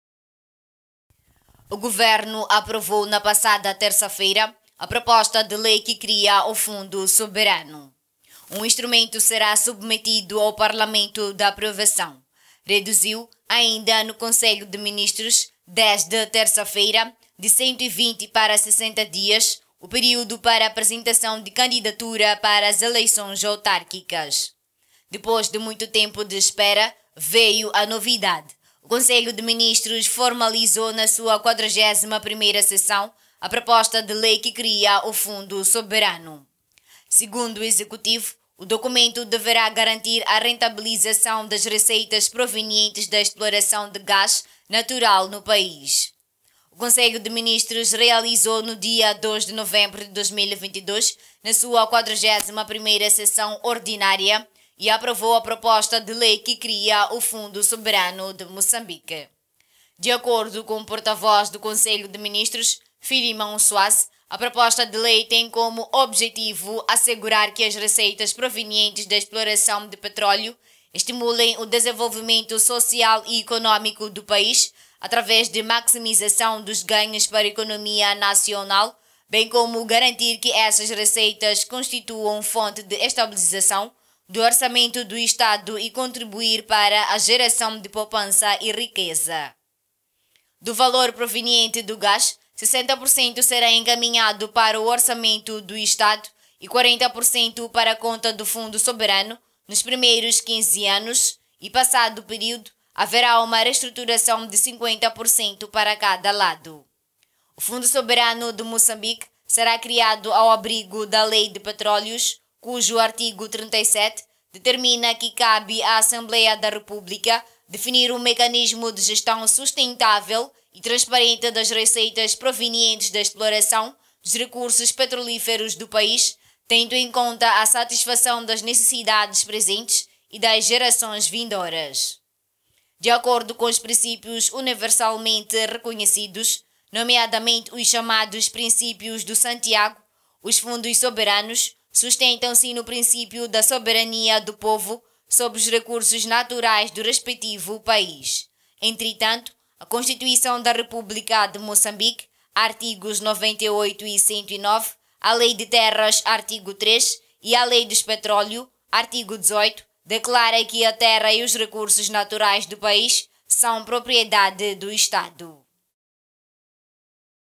GOVERNO APROVA LEI DE FUNDO SOBERANO
O Governo aprovou, na passada terça-feira, a proposta de lei que cria o Fundo Soberano. O instrumento será submetido ao Parlamento para aprovação. Reduziu, ainda no Conselho de Ministros, desde terça-feira, de 120 para 60 dias o período para apresentação de candidatura para as eleições autárquicas.